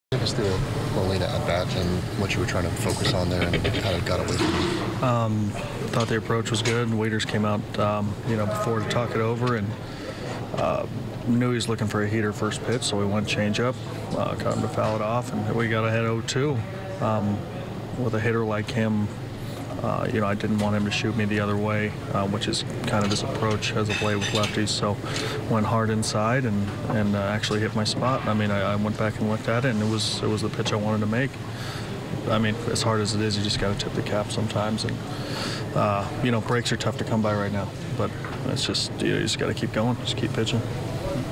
Sammy Solís on four-run inning vs. Cardinals
Sammy Solís gave up a grand slam to Yadier Molina in the ninth inning of the Nationals' 11-8 loss to the Cardinals.